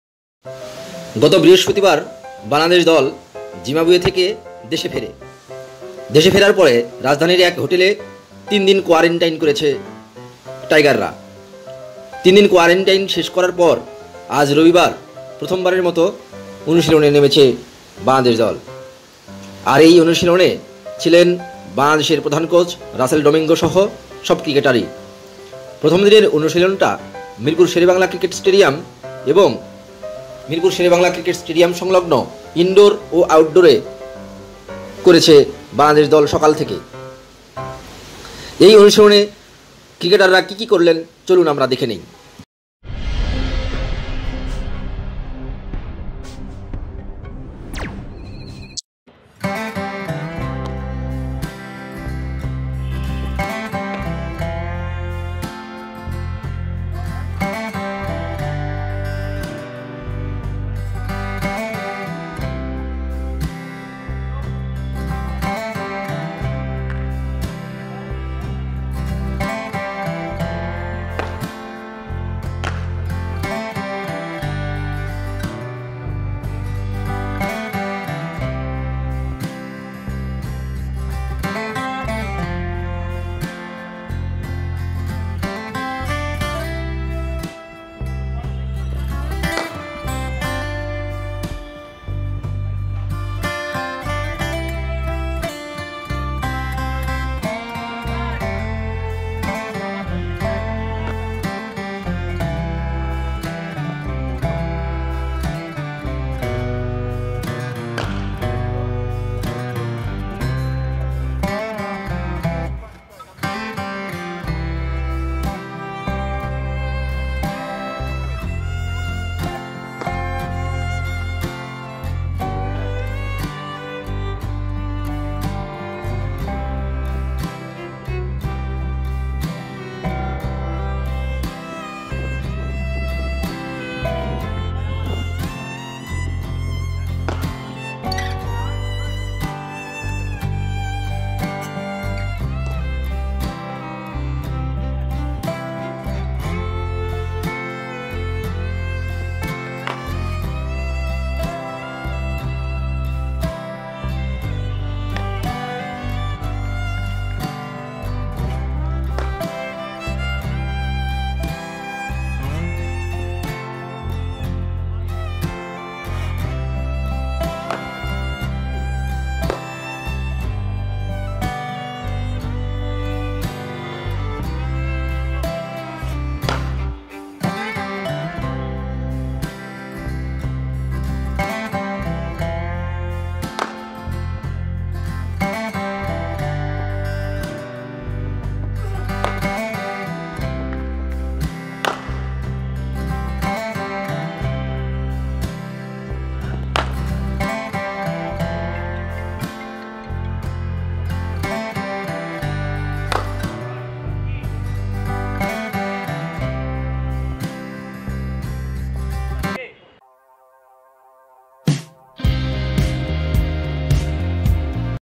অস্ট্রেলিয়ার জন্য প্রস্তুত হচ্ছে টাইগাররা- BD Team is Preparing for 5-match T20 Series vs AUS.
৩ আগস্ট থেকে শুরু হবে বাংলাদেশ-অস্ট্রেলিয়া ৫ ম্যাচের টি২০ সিরিজ। এই সিরিজকে সামনে রেখে আজ (১ আগস্ট) বাংলাদেশ দল অনুশীলন শুরু করেছে। জিম্বাবুয়ে থেকে ফিরে ৩ দিনের হোটেল কোয়ারেন্টাইন শেষ করে আজই ছিল টাইগারদের প্রথম অনুশীলন।